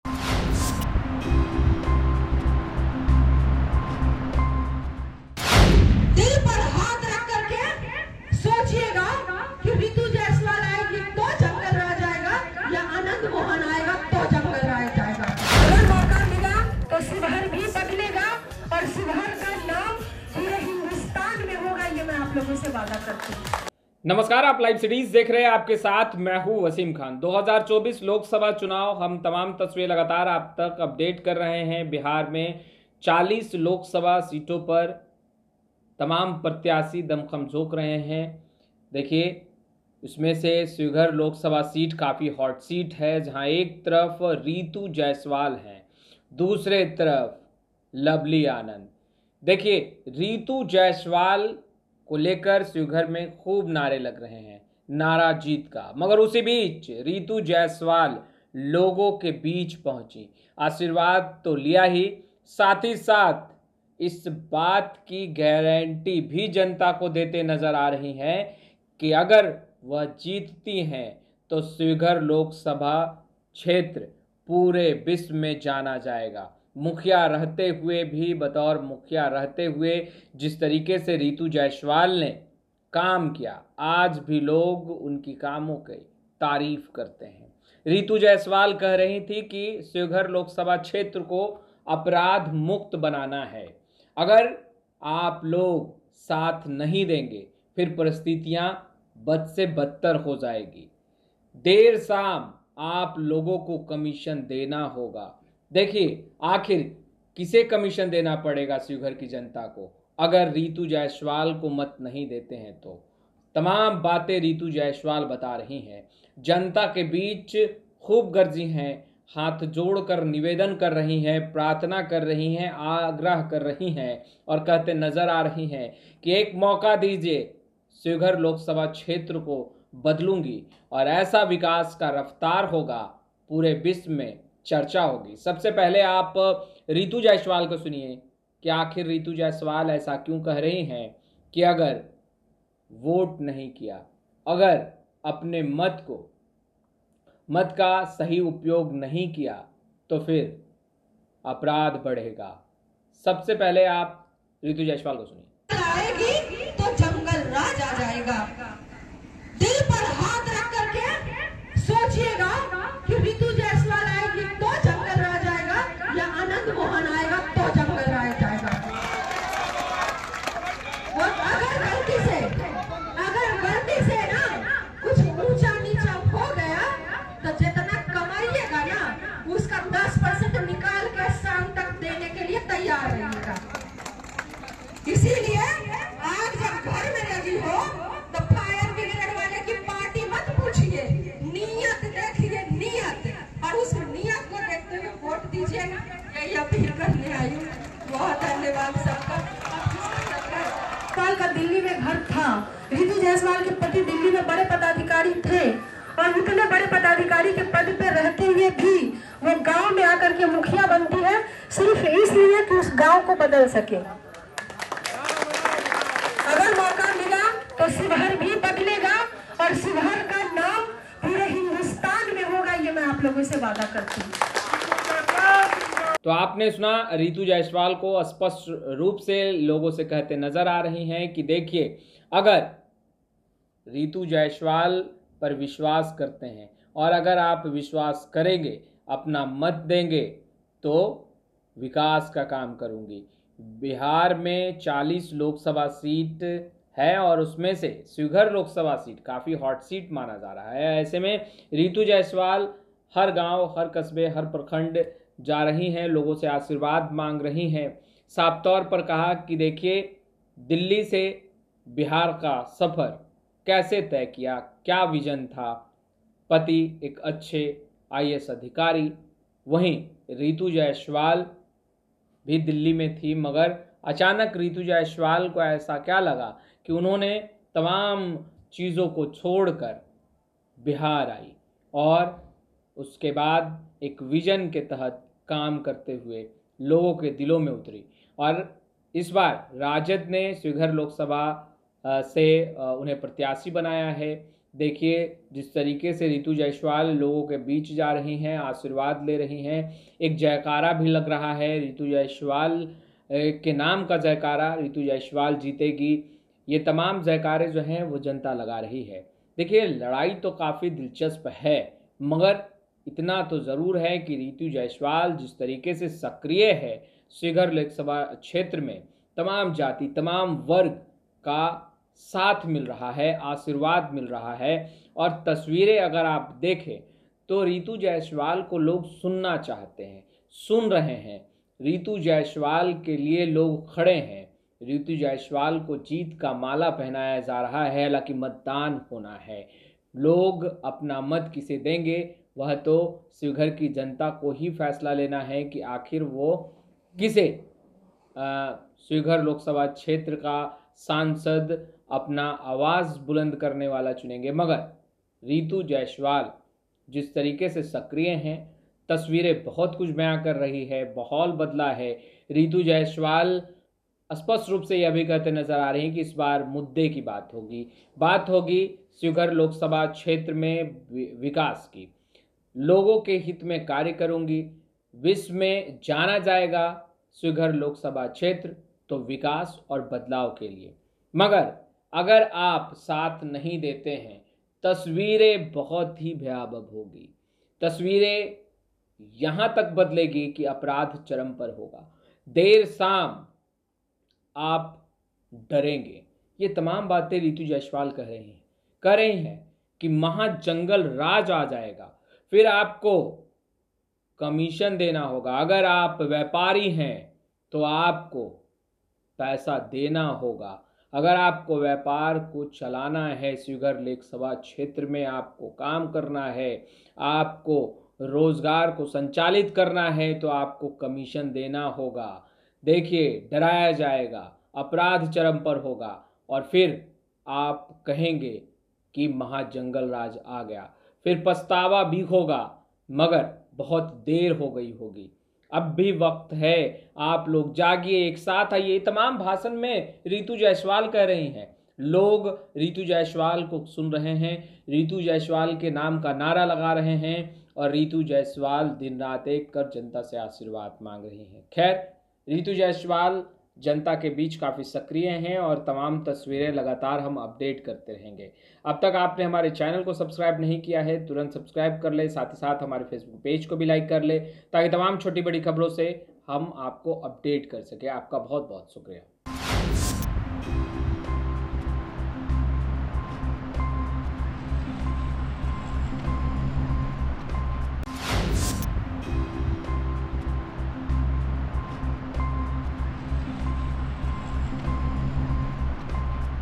Sheohar Lok Sabha में जनता के बीच खूब गरजीं Ritu Jaiswal, बड़ा ऐलान भी, जल्द बदलेगी तस्वीर...
Sheohar Lok Sabha में जनता के बीच खूब गरजीं Ritu Jaiswal, बड़ा ऐलान भी, जल्द बदलेगी तस्वीर... | Bihar News | Livecities